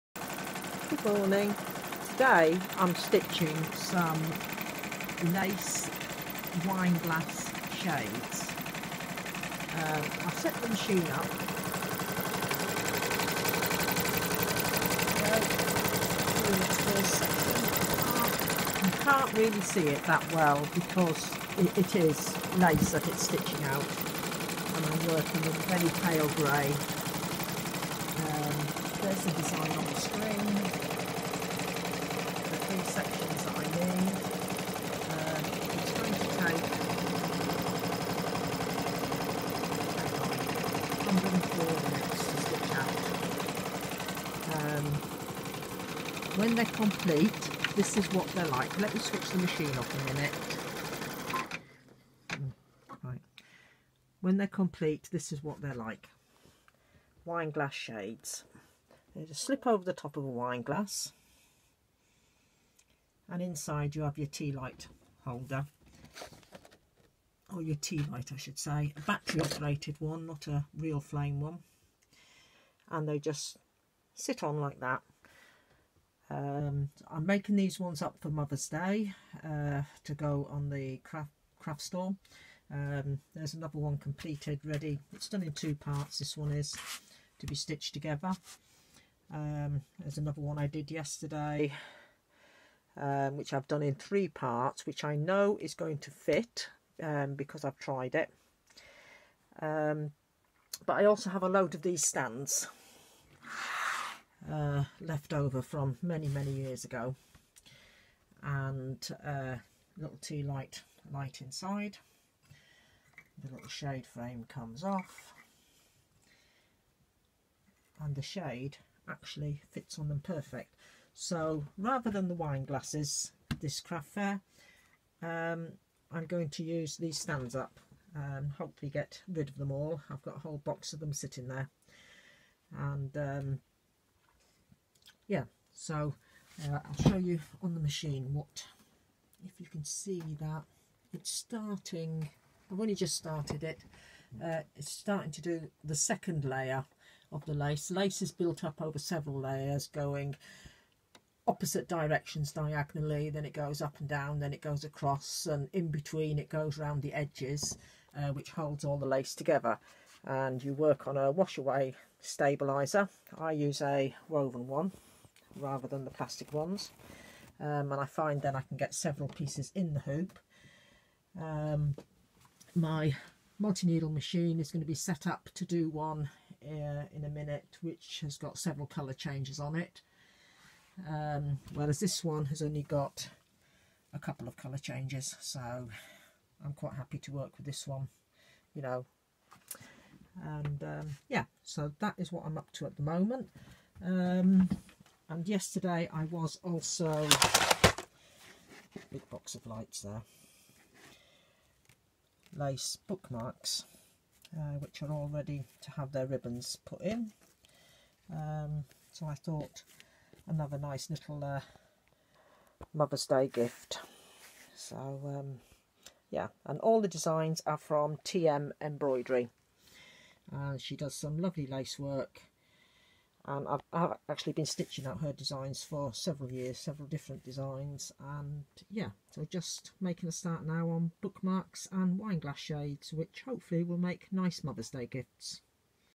Lace work on the Brother V7
Doing a bit of lace work a mix of bookmarks and led candle shades